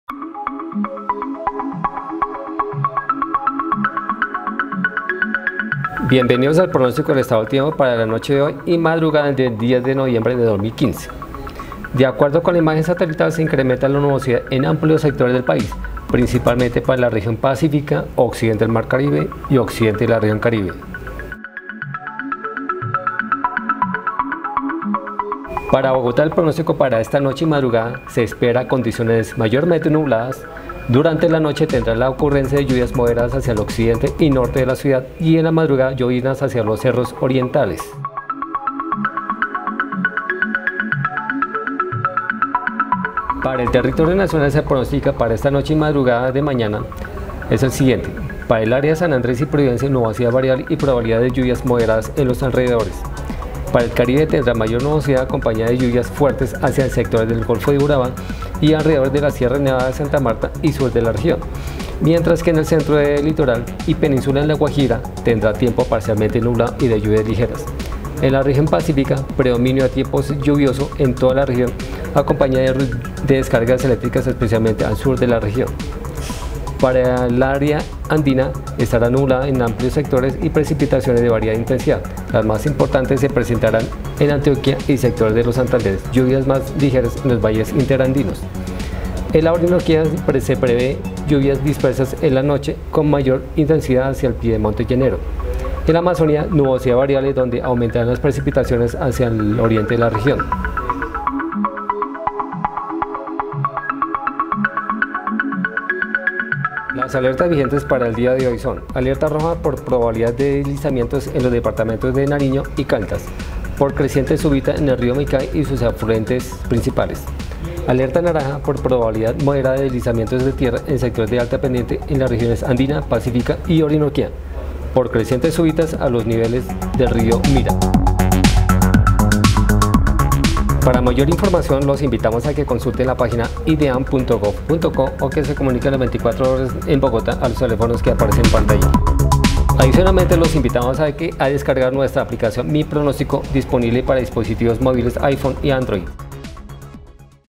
Noche del Lunes 09 y Madrugada del Martes 10 de Noviembre de 2015.
Pronóstico del tiempo virtual para la Noche del  Lunes 09 y Madrugada del Martes 10 de Noviembre de 2015.. anunciando condiciones Hidrometeorológicas en Colombia.